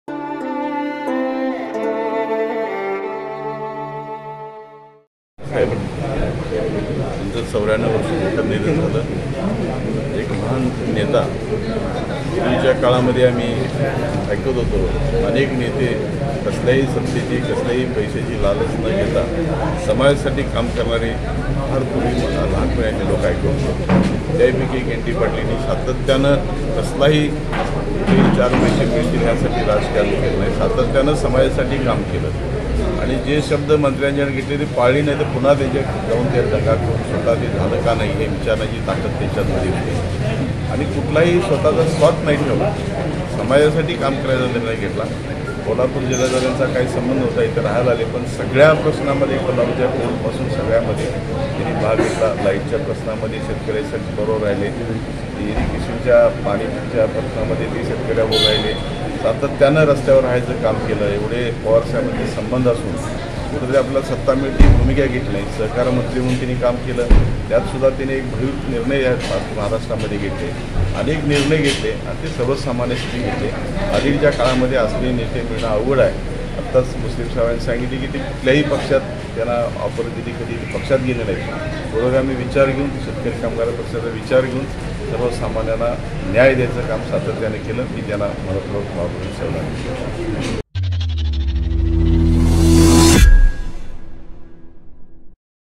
एन. डी. पाटलांसारखे नेते मिळणे अवघड : आ. पी. एन. पाटील
दिवंगत ज्येष्ठ नेते डॉ. एन. डी. पाटील यांच्याविषयी भावना व्यक्त करून आ. पी. एन. पाटील यांनी त्यांना श्रध्दांजली वाहिली.